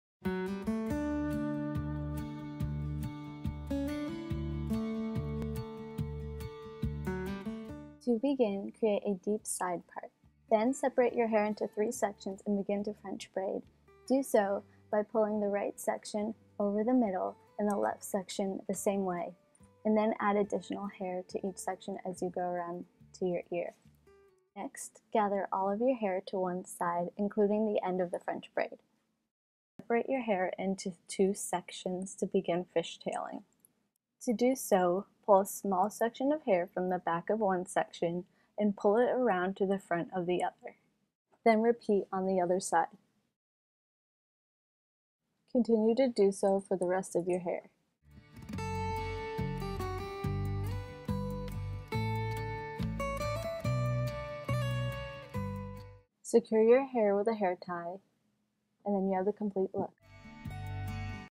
Get Fairytale Hair: How to Do a French and Fishtail Braid | Parents
Looking for a fun and easy hairstyle to try? In this video, we give you step-by-step instructions to do a French and fishtail braid.